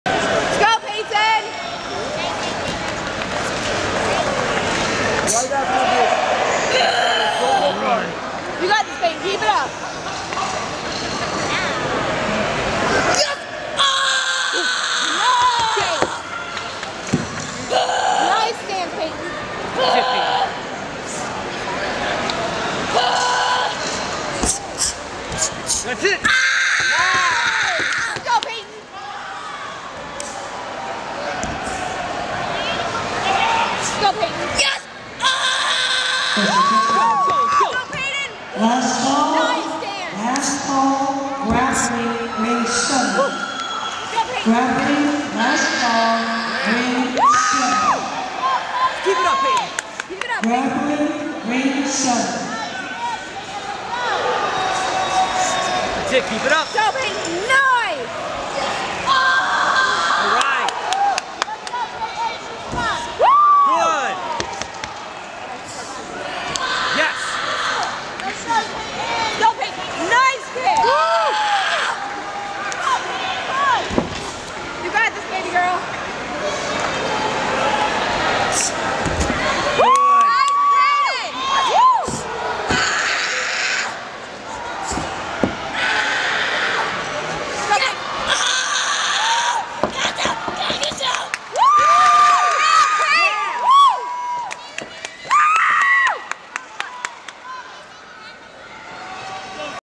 Peyton Swatek - 2012 Gator Nationals - Traditional Form
Another newcomer who made it to stage at the Gators was Swatek with a strong traditional performance in the 13 & Under girls traditional forms grands.